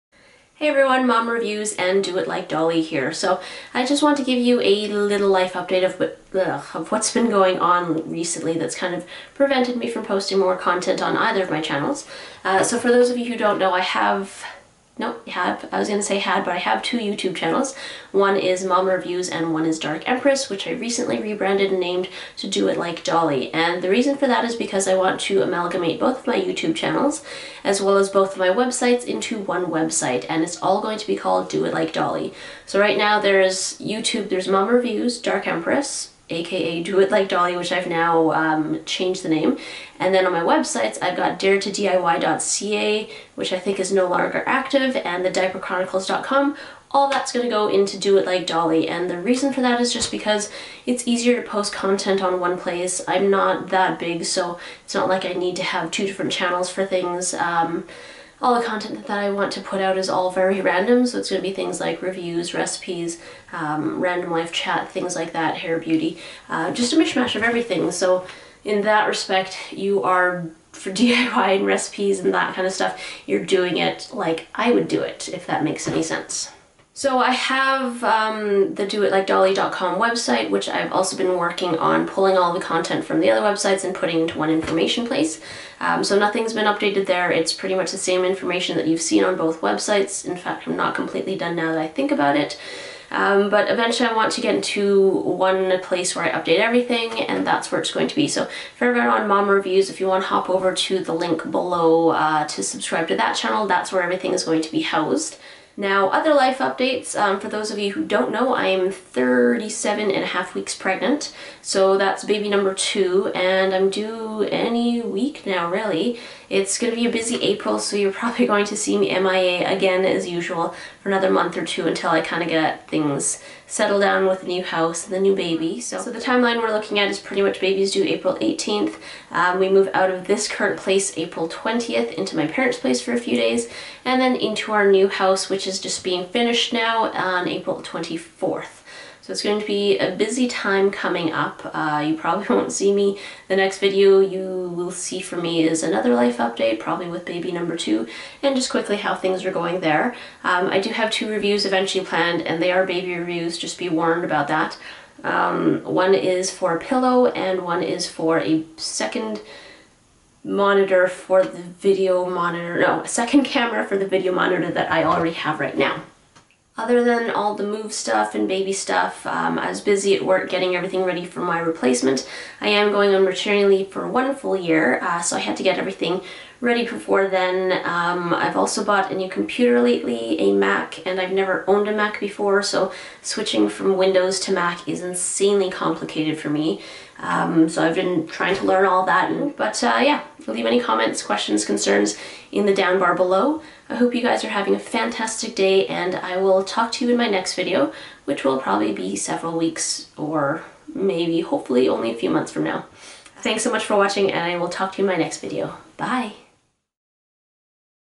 Another Life Update!
So remember how I said I would be able to do more videos after finishing my wood working projects? Well...ya...I lied :p Life got in the way! Quick update before my baby arrives (any week now) and just a heads up that you should pop over to my other channel to subscribe as that will become my main (and only) channel where I post my videos :)

LINKS